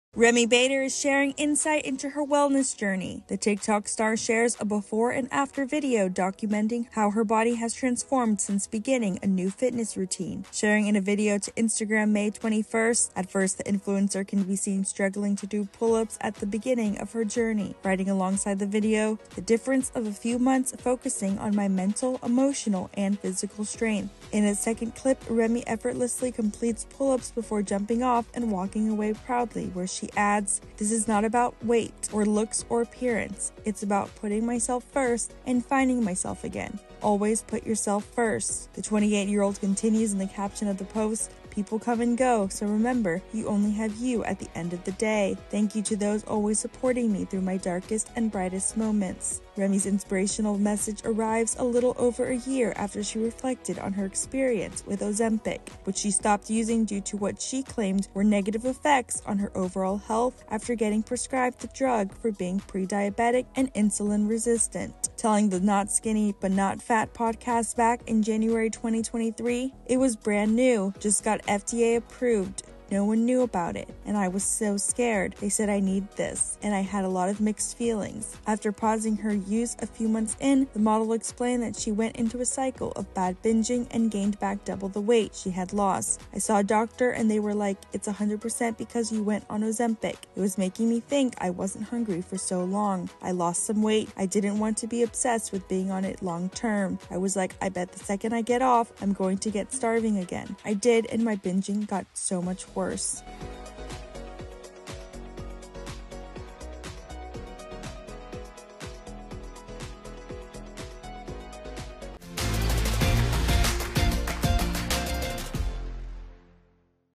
TikToker Remi Bader REVEALS Before and After Fitness Transformation | E! News
TikTok star Remi Bader recently shared before-and-after videos of her fitness journey, urging others to “put yourself first.” This comes a year after she got candid about her weight loss journey.